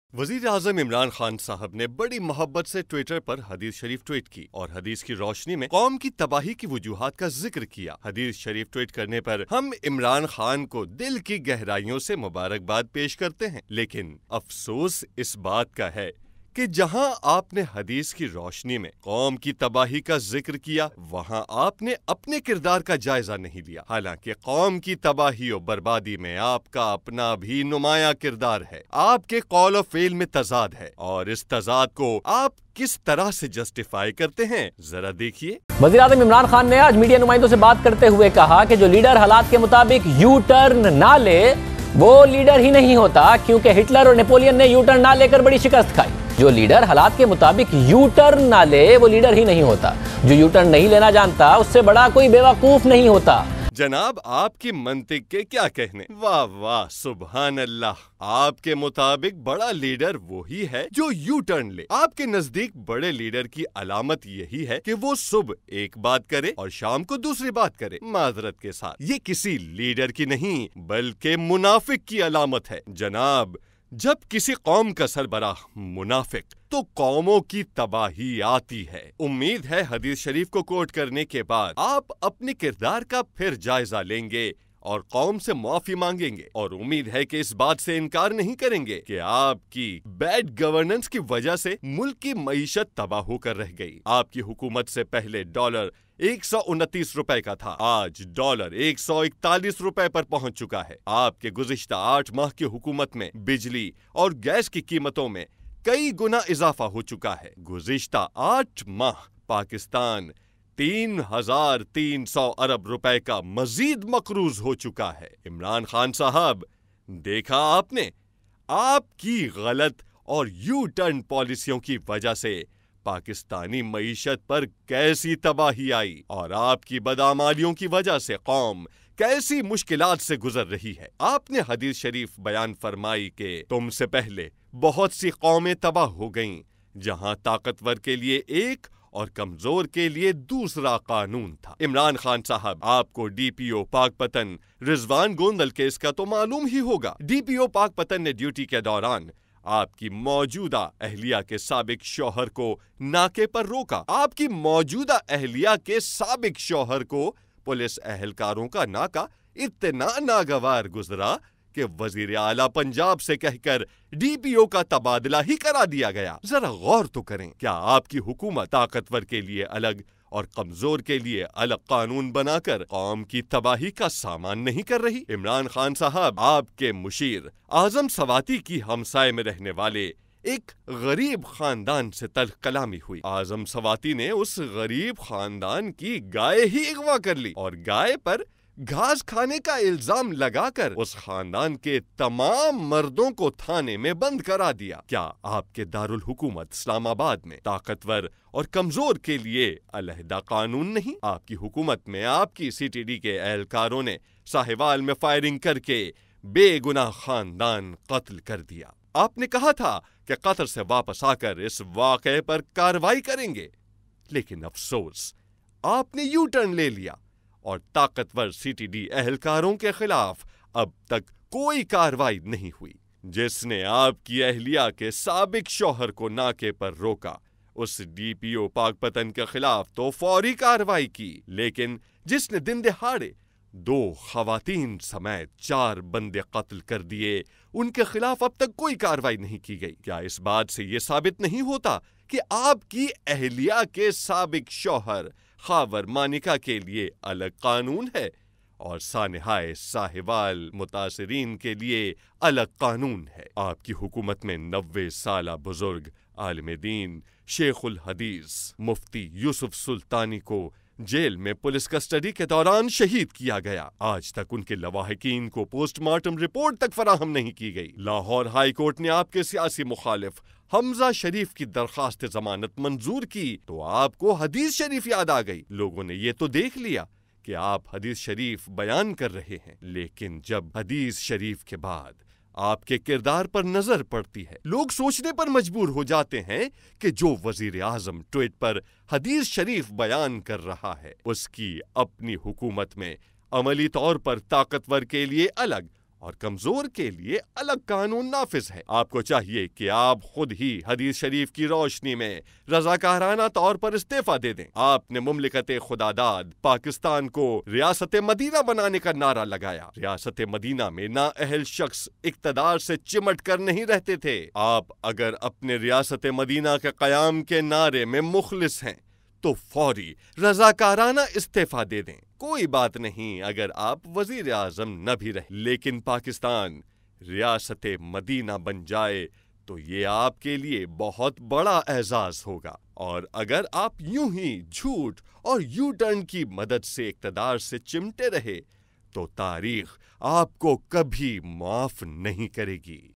Excellent Reply to tweet of PM Imran Khan by Labbaik TV | TLP
وزیراعظم عمران خان کی ٹوئٹ پر لبیک ٹی وی نے  عمران خان کو دن میں تارے دکھا دیئے

لبیک ٹی وی کی جانب سے عمران خان کو مدلل جواب